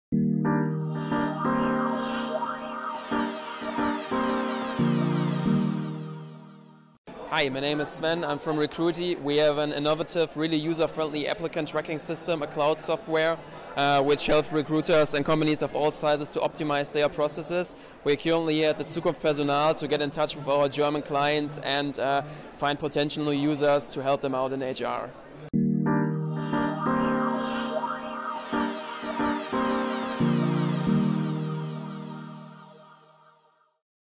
Sven Ehlhardt, Account Executive, recruitee at ZPEurope18
Recruitee is the easiest way for teams to organize their hiring process - and grow faster.